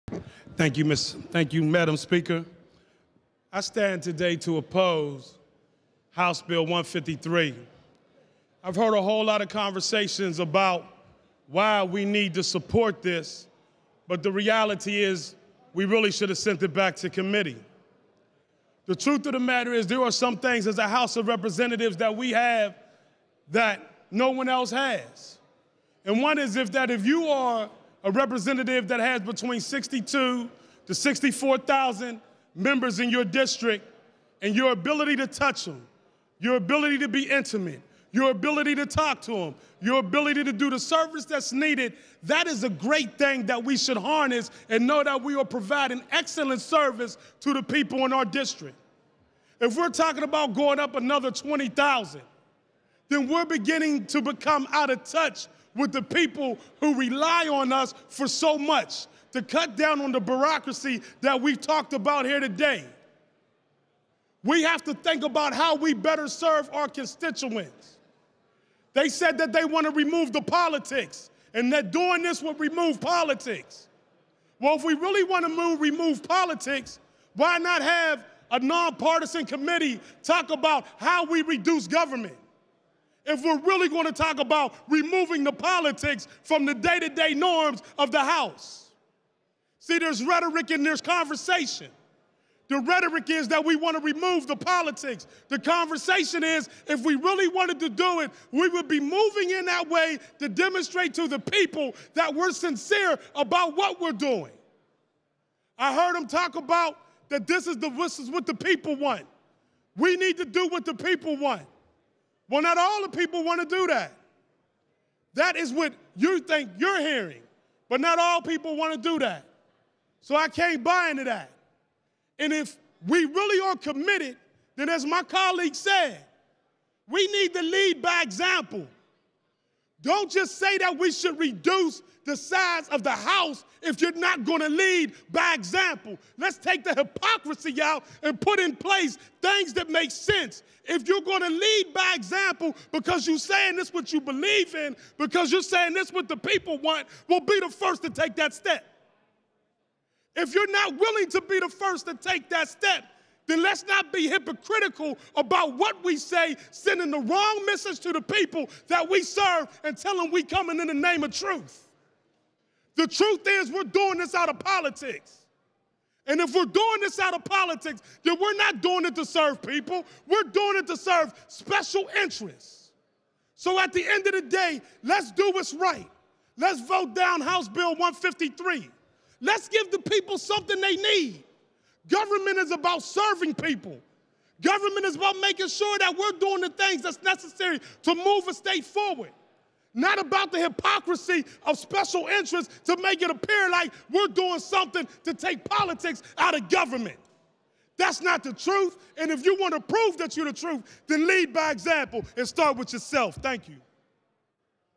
Gainey: Don't Change District Size
Pa. state Rep. Ed Gainey speaks in favor keeping legislative districts at their current size. Gainey reducing the legislature will negatively affect constituent service and is not meaningful government reform.